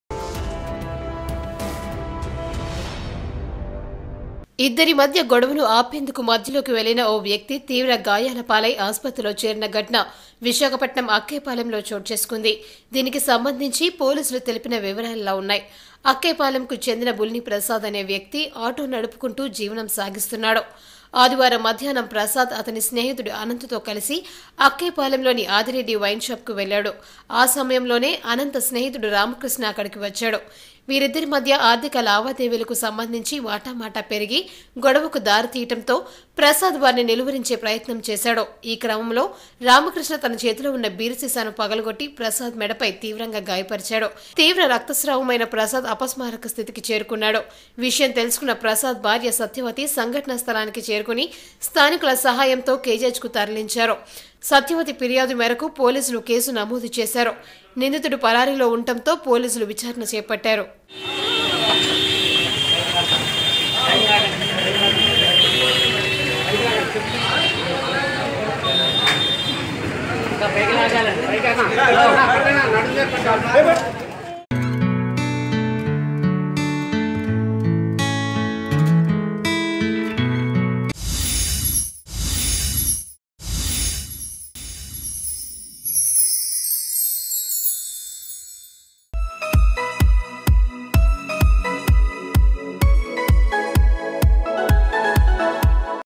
Man Injured Seriously in Clash Between Two Persons || Visakhapatnam
AMPM Live : ఇద్దరి మధ్య గొడవను ఆపేందుకు మధ్యలోకి వెళ్లిన ఓ వ్యక్తి తీవ్ర గాయాలపాలై ఆసుపత్రిలో చేరిన ఘటన విశాఖపట్టణం అక్కయ్యపాలెంలో చోటుచేసుకుంది. దీనికి సంబంధించి పోలీసులు తెలిపిన వివరాలు ఉన్నాయి. అక్కయ్యపాలెంకు చెందిన బులిని ప్రసాదు (33) అనే వ్యక్తి ఆటో నడుపుకుంటూ జీవనం సాగిస్తున్నాడు. ఆదివారం మధ్యాహ్నo ప్రసాద్ అతని స్నేహితుడు అనంతతో కలిసి అక్కయ్యపాలెంలోని ఆదిరెడ్డి వైన్ షాప్ కు వెళ్లాడు. ఆ సమయంలోనే అనంత స్నేహితుడు రామకృష్ణ అక్కడికి వచ్చాడు. వీరిద్దరి మధ్య ఆర్ధిక లావాదేవీలకు సంబంధించి మాటామాటా పెరిగి గొడవకు దారి తీయడంతో ప్రసాద్ వారిని నిలువరించే ప్రయత్నం చేసాడు. ఈ క్రమంలో రామకృష్ణ తన చేతిలో ఉన్న బీరు సీసాను పగలగొట్టి ప్రసాద్ మెడ పై తీవ్రంగా గాయపరిచాడు. తీవ్ర రక్తస్రావమైన ప్రసాద్ అపస్మారక స్థితికి చేరుకున్నాడు. విషయం తెలుసుకున్న ప్రసాద్ భార్య సత్యవతి సంఘటన స్థలానికి చేరుకొని స్థానికుల సహాయంతో కేజీహెచ్ కు తరలించారు. సత్యవతి ఫిర్యాదు మేరకు పోలీసులు కేసు నమోదు చేసారు. నిందితుడు పరారీలో ఉండడంతో పోలీసులు విచారణ చేపట్టారు.